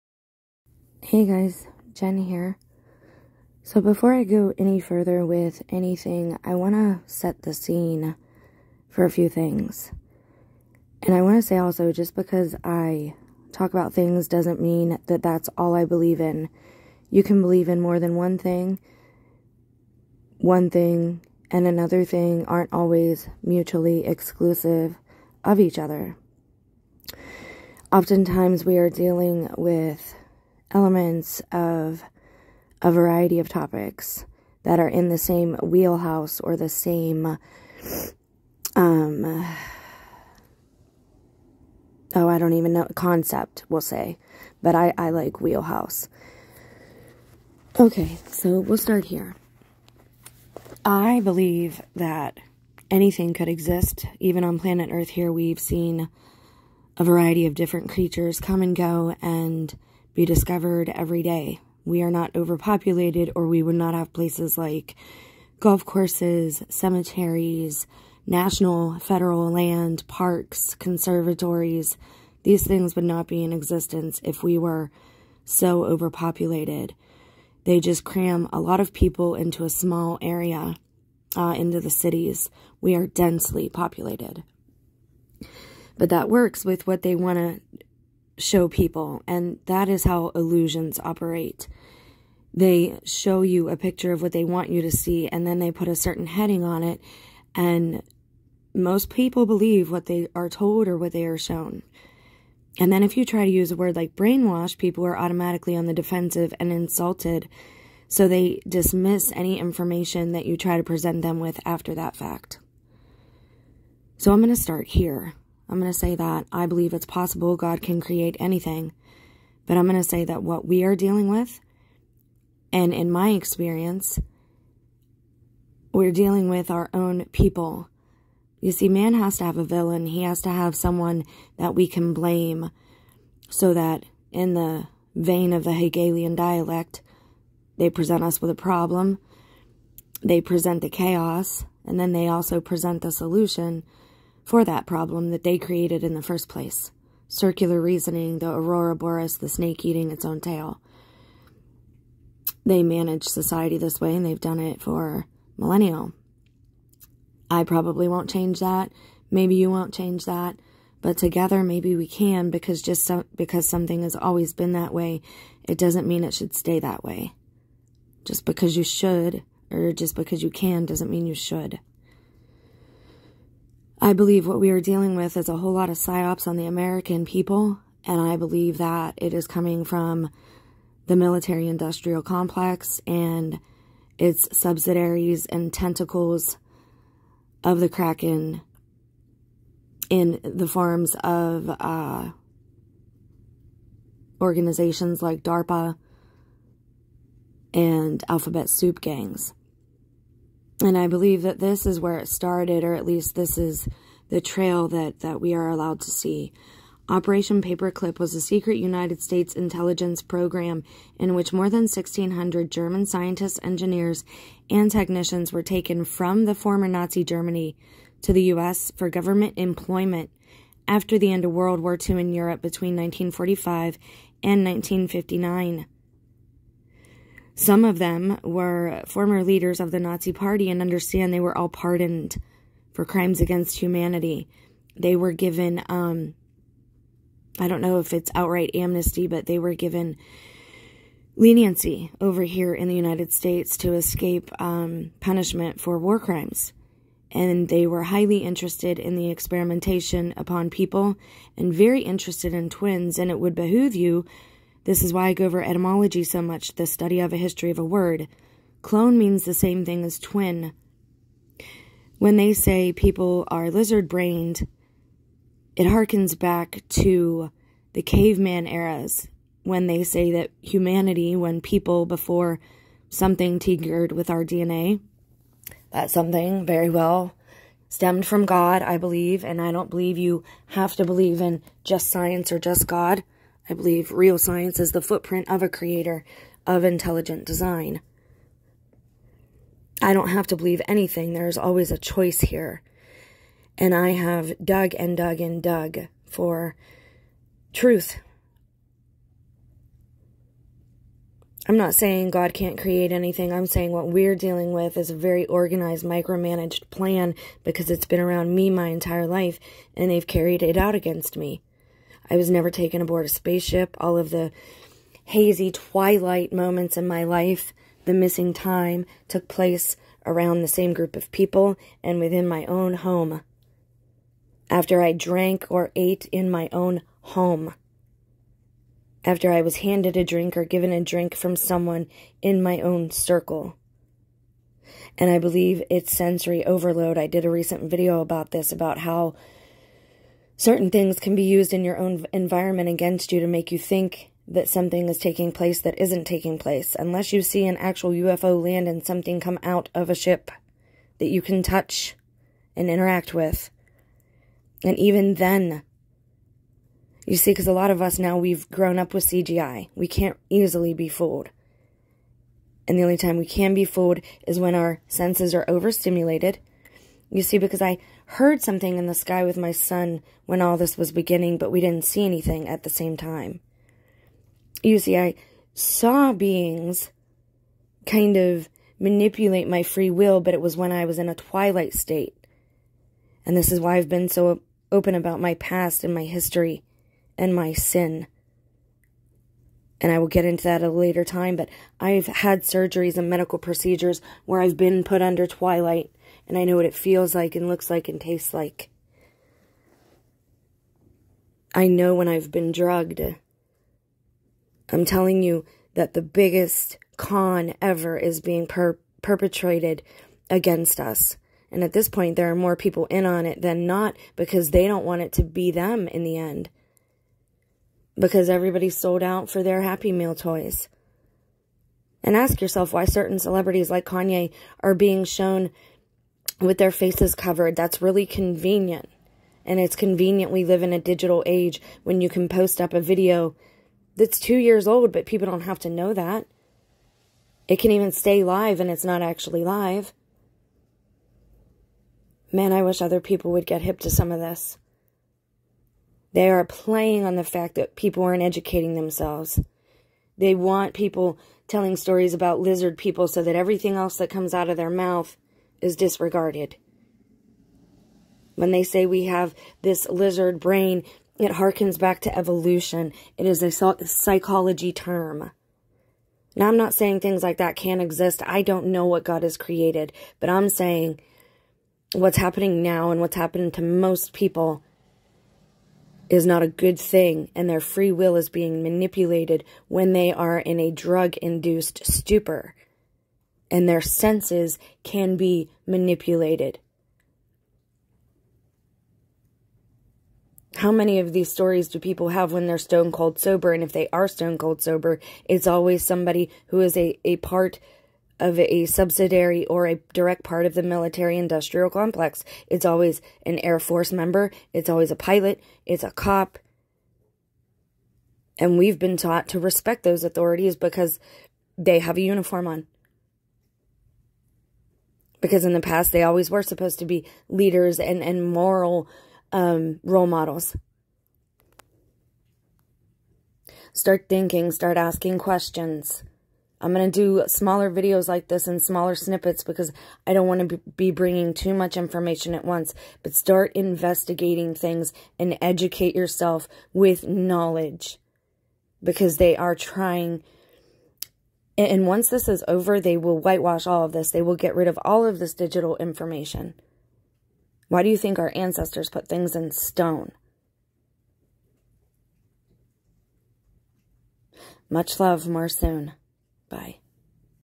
The Military Industrial Complex + Project Paperclip = Creating Villians
They need to present us with a common enemy so that there isn't s public outcry fir thr elites to be lynched for their crimes against humanity but especially the children. always read description and pinned comments for further explanation of videos.